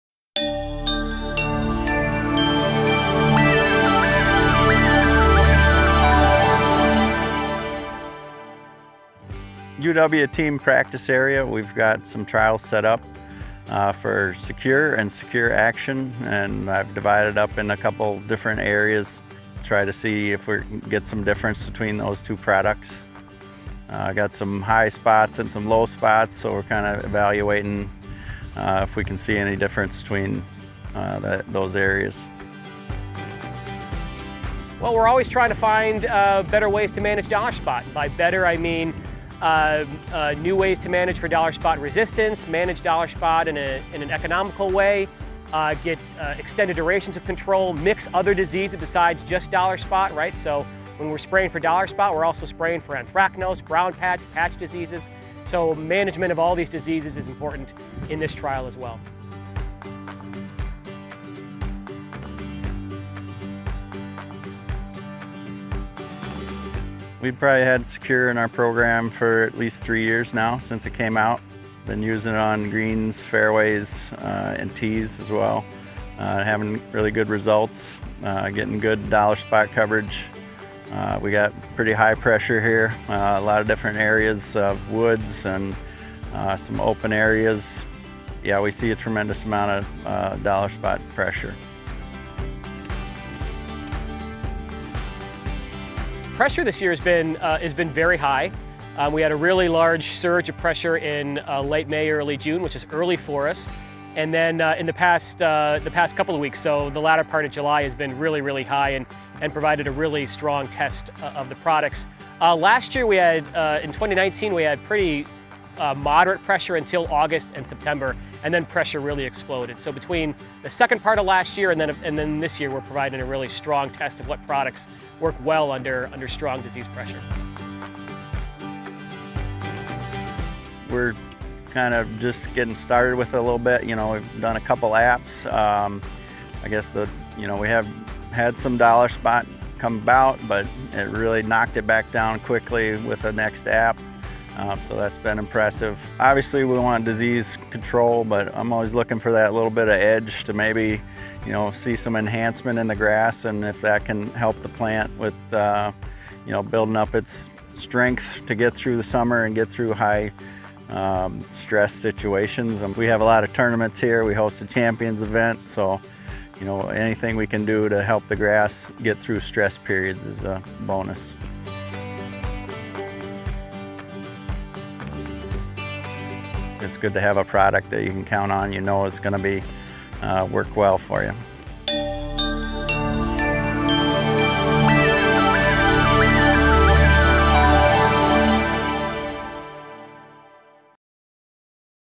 Disease Control During High Pressure at UW-Madison
University Ridge at the University of Wisconsin-Madison has set up their team trials area for testing to see the difference between Secure and Secure Action as Dr. Paul Koch, Associate Professor of Plant Pathology at the University of Wisconsin-Madison tells us this year has yielded a strong test of products due to high disease pressure in the area.